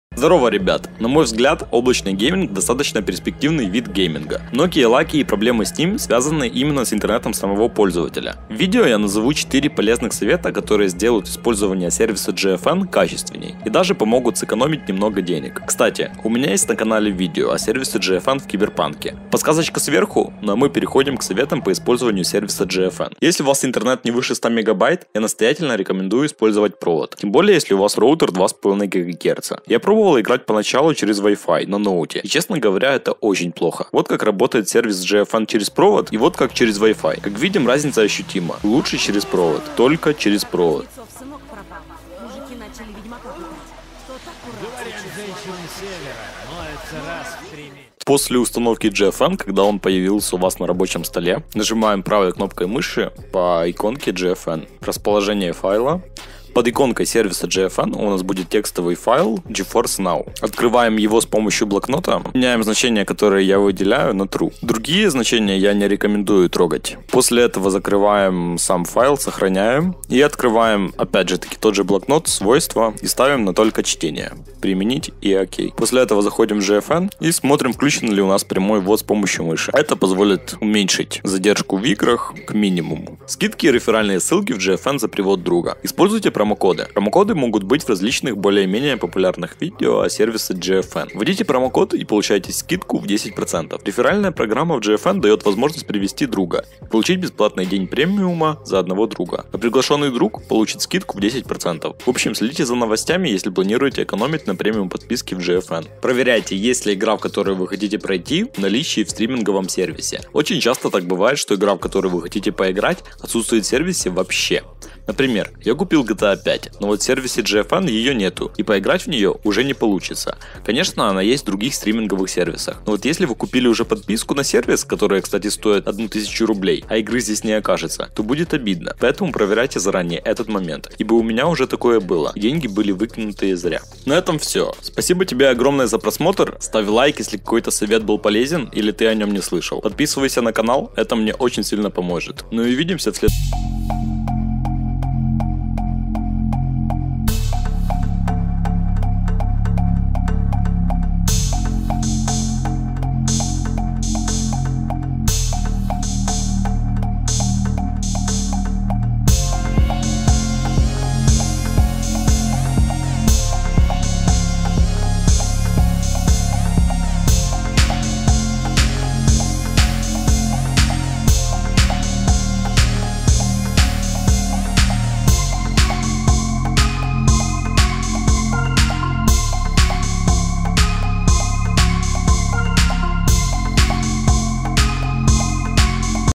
4 СОВЕТА КОТОРЫЕ СДЕЛАЮТ ИСПОЛЬЗОВАНИЕ GFN // GEFORCE NOW ЛУЧШЕ / БЕЗ ВОДЫ
ВИДЕО О ОПТИМИЗАЦИИ И УЛУЧШЕНИИ РОБОТЫ СЕРВИСА GFN// GEFORCE NOW.
Таймкоды:
 0:33 Используйте проводной интернет
0:50 Уменьшение задержки мыши и клавиатуры
1:28 Економьте на GFN
1:48 Проверяйте наличие игр перед покупкой подписки